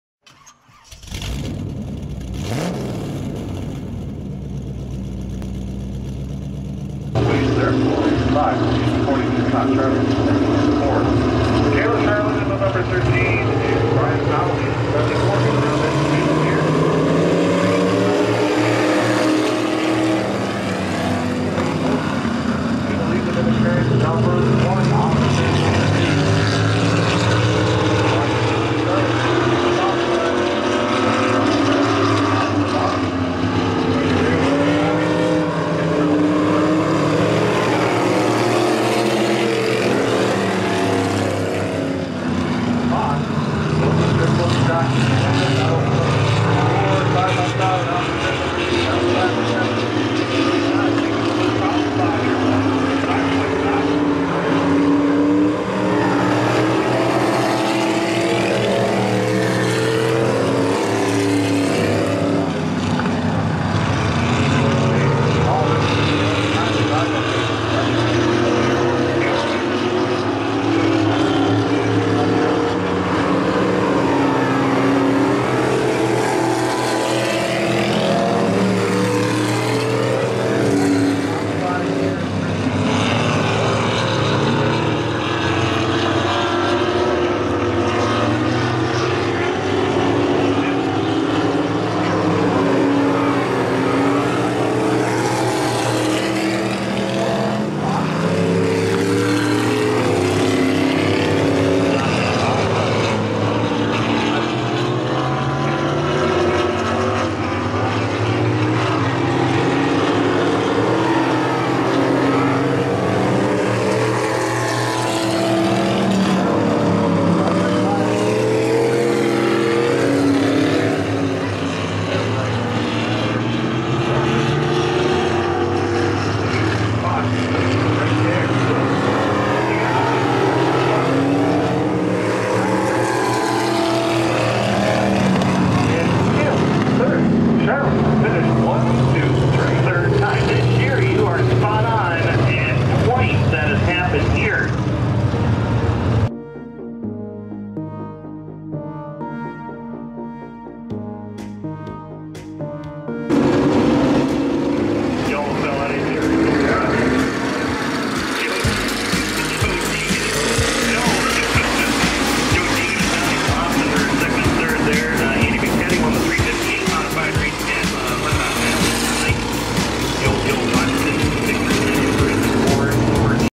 Vintage Modified Heat 3 August 15 Brockville Speedway
Heat 3 of 3 for the Eastern Ontario Vintage Stock Car Club August 15, 2020 @ Brockville Speedway.

Like Martys Motorized Mayhem on Facebook.